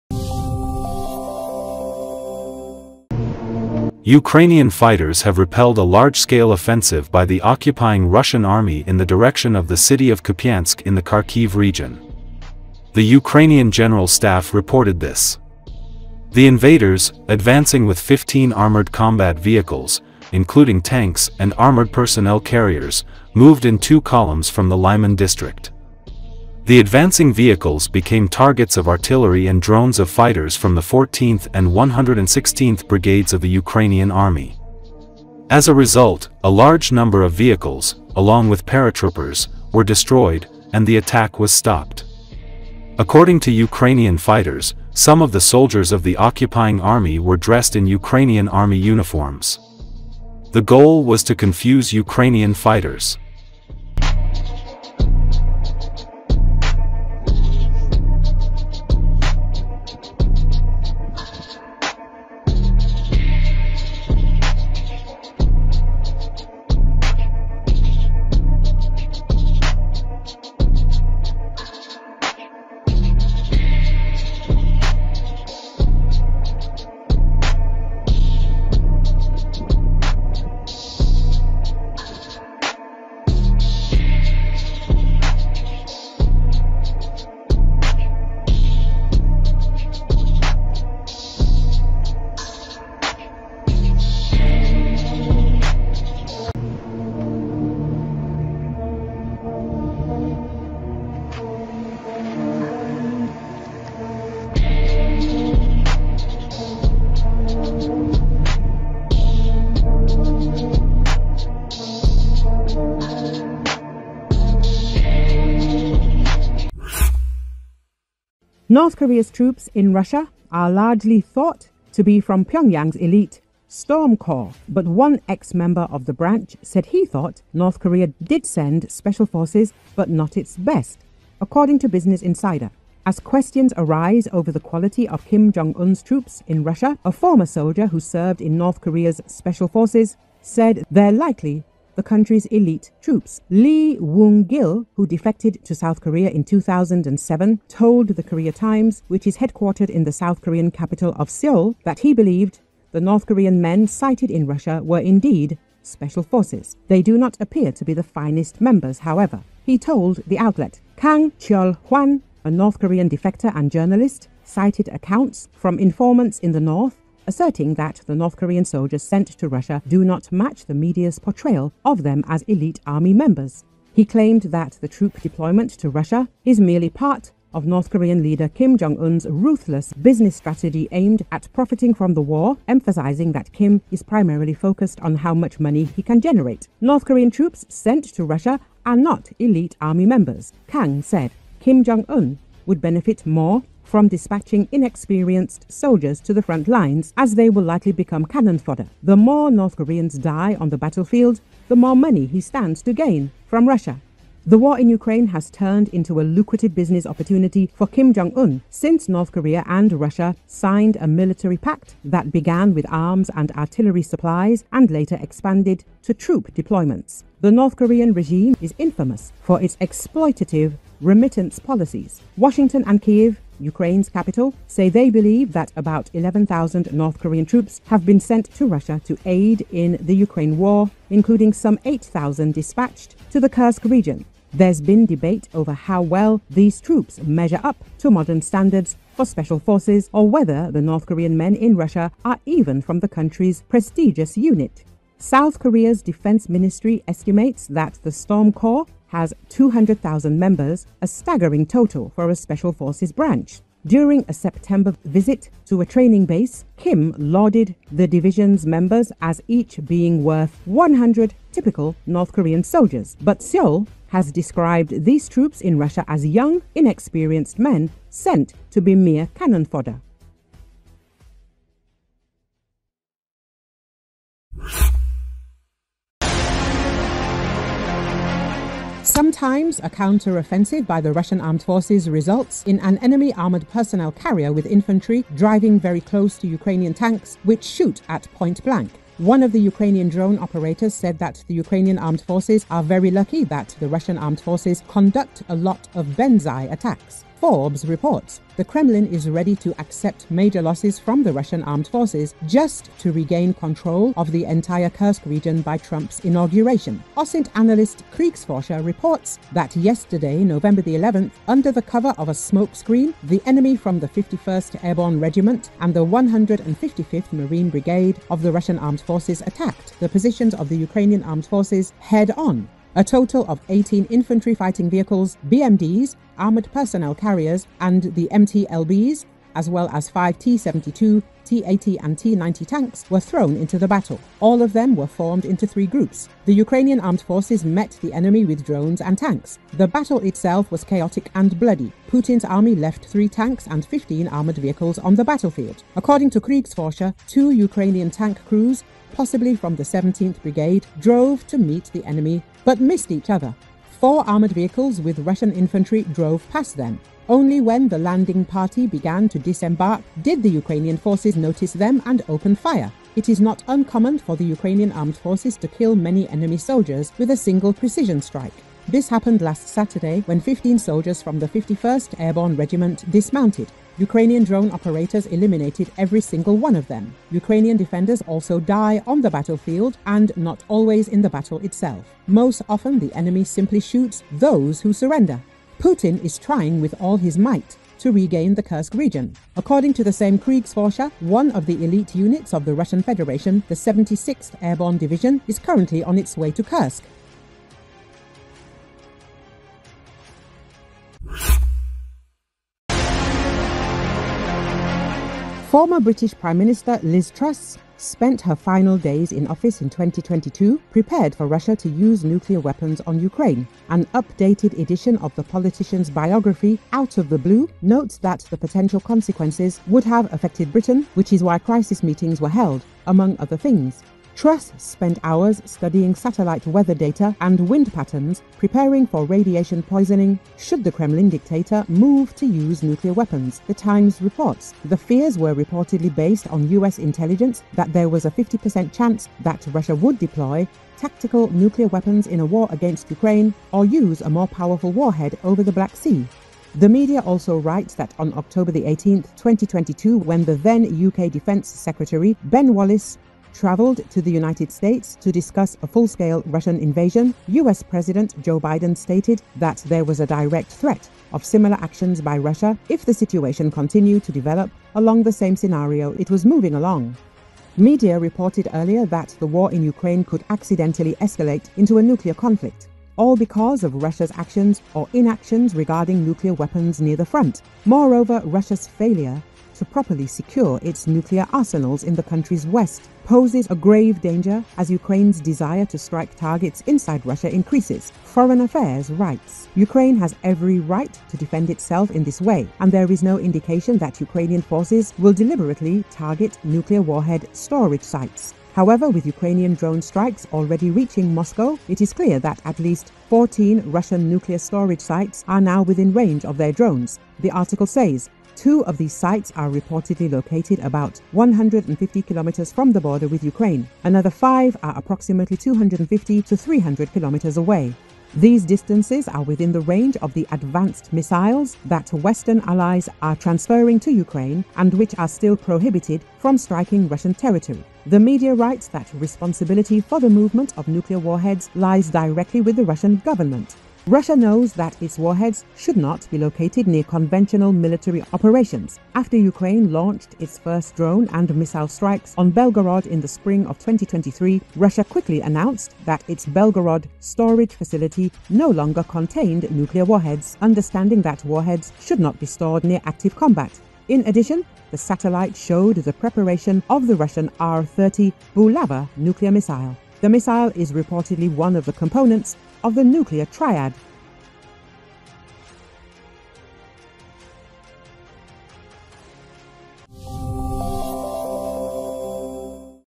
The brigade of Russians attacking Kupyansk with 15 equipment, was targeted by artillery and drones
Ukrainian fighters have repelled a large-scale offensive by the occupying Russian army in the direction of the city of Kupyansk in the Kharkiv region. The Ukrainian General Staff reported this. The invaders, advancing with 15 armored combat vehicles, including tanks and armored personnel carriers, moved in two columns from the Liman district. The advancing vehicles became targets of artillery and drones of fighters from the 14th and 116th brigades of the Ukrainian army. As a result, a large number of vehicles, along with paratroopers, were destroyed, and the attack was stopped. According to Ukrainian fighters, some of the soldiers of the occupying army were dressed in Ukrainian army uniforms. The goal was to confuse Ukrainian fighters.